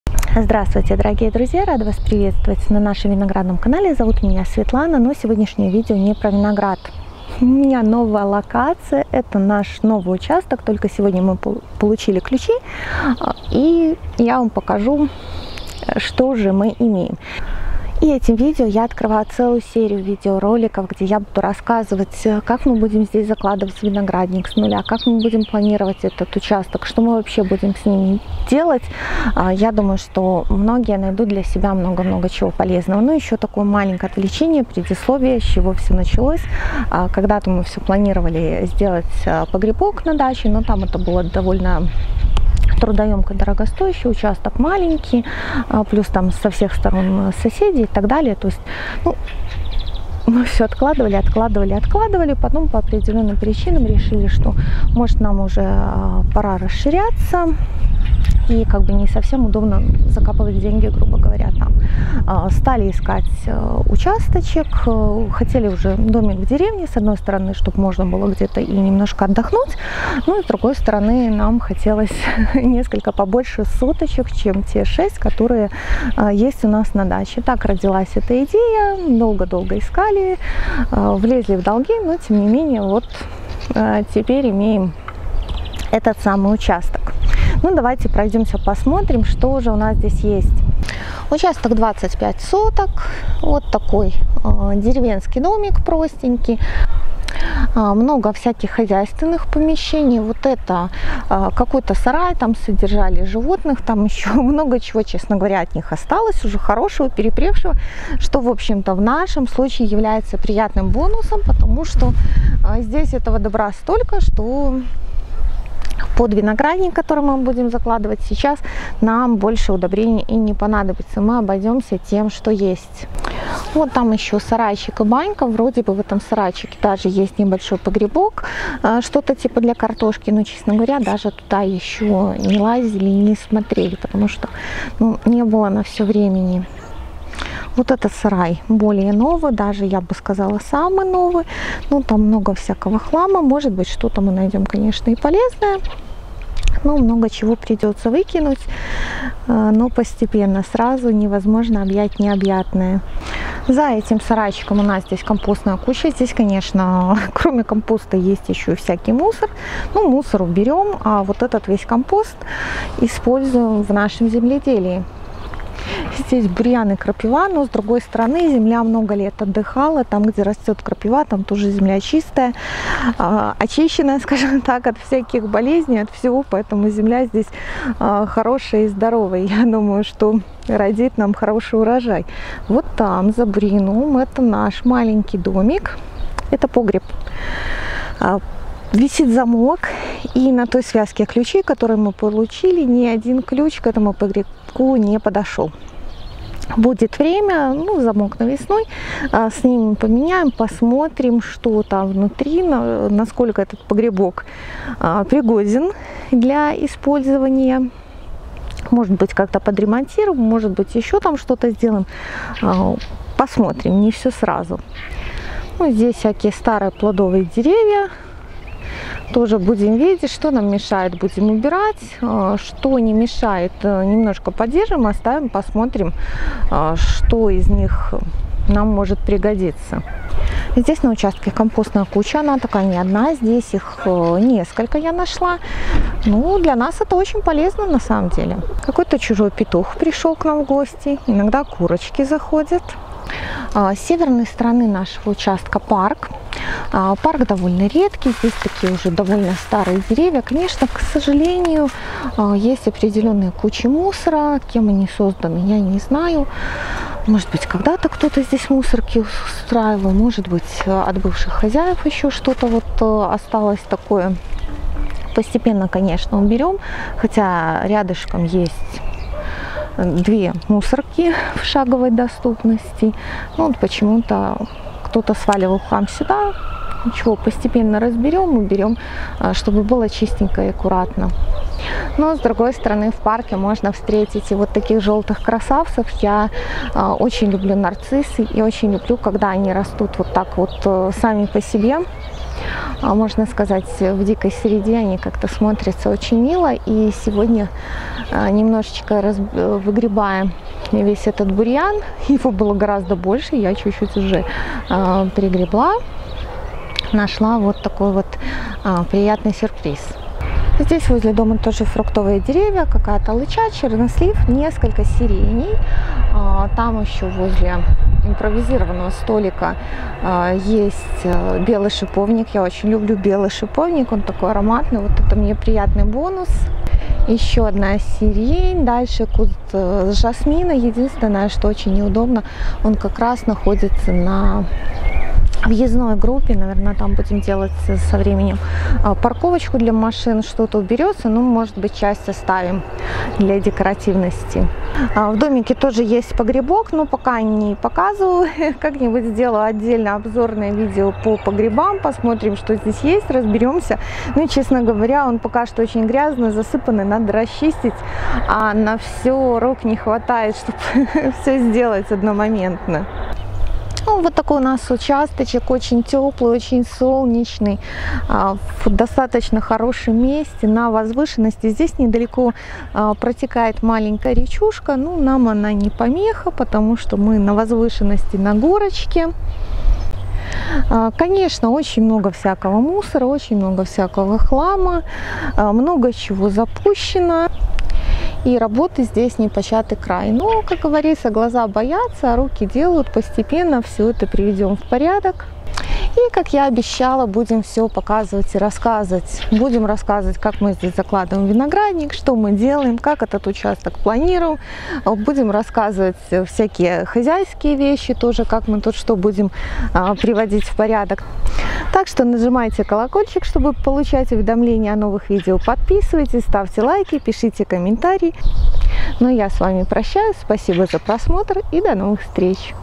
Купили участок под виноградник и не только. Обзор владений
Расширяем виноградник, купили участок для посадок и для жизни, показываем его вам.

Карта (перевод на карту иностранного банка либо через систему paysend.com)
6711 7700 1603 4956
Varabyova Sviatlana
 WebMoney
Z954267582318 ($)